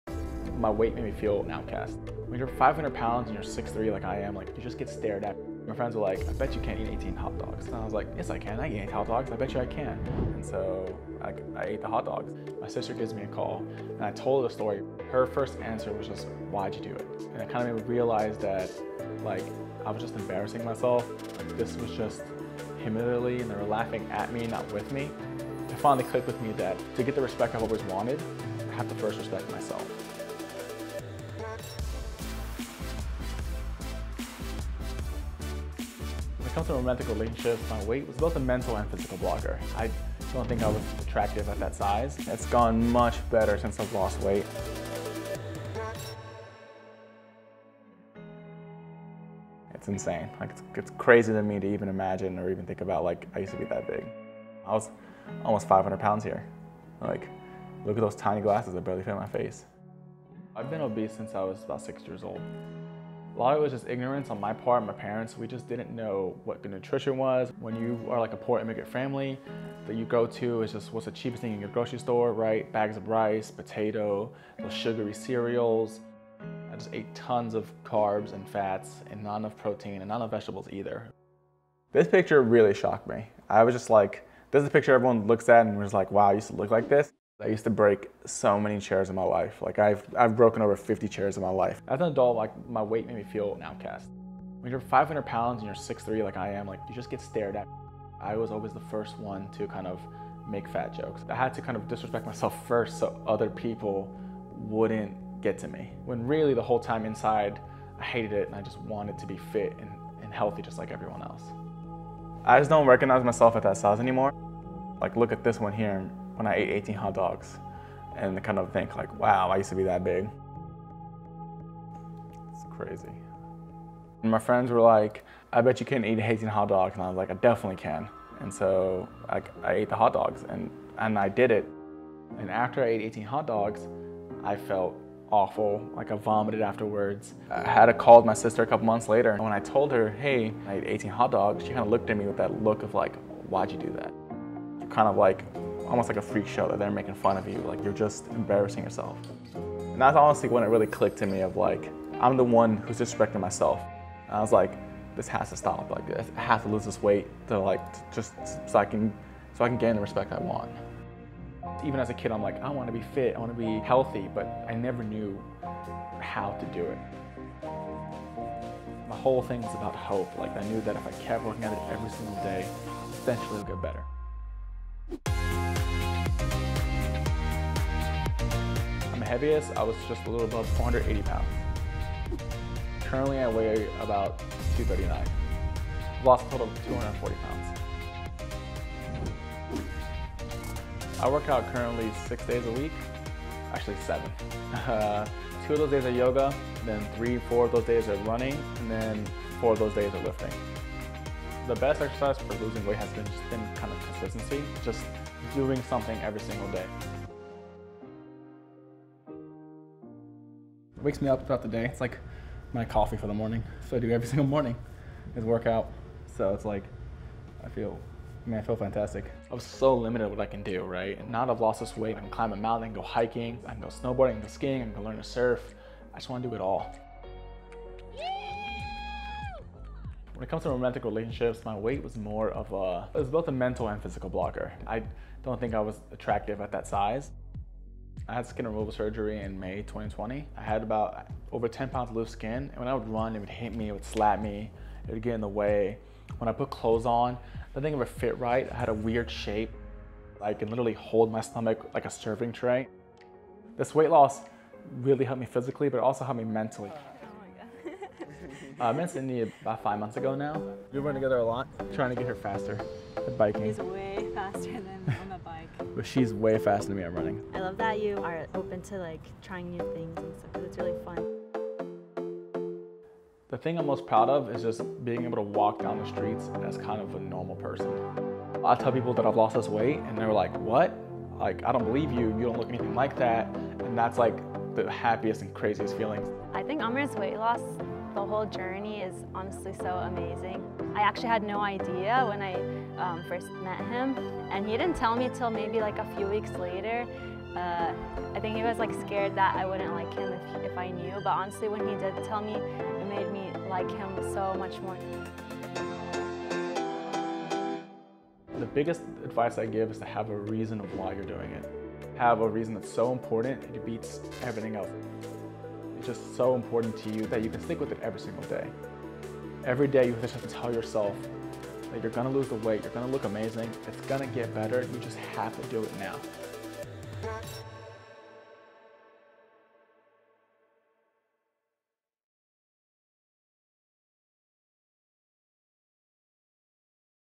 I Ate 18 Hot Dogs At 500lbs - Look At Me Now | BRAND NEW ME
AFTER eating 18 hot dogs in one sitting to win a bet, Amer Ismail found the motivation to lose almost half his body weight. Amer, 27, has struggled with weight most of his life, weighing almost 470lbs at his heaviest. Ismail, of Houston, Texas, managed to lose 241lbs after realising he couldn’t continue living the way he was.

Video Credits:
Videographer / director:  Davin Fitch
Producer: Martha Hewett
Editor: Sarah-Jayne Galpin